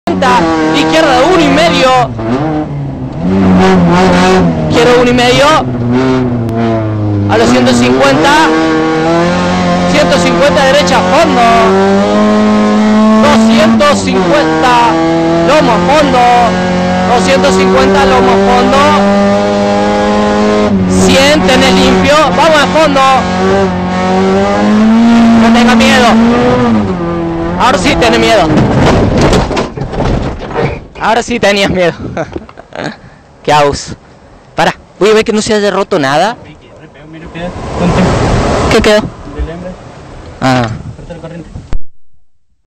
Facu Salgado Nash Accidente Rally San Pedro 2018
Rally Regional Cordobés
PE: Los Callejones - San Isidro
Fiat Palio N1
SN Competición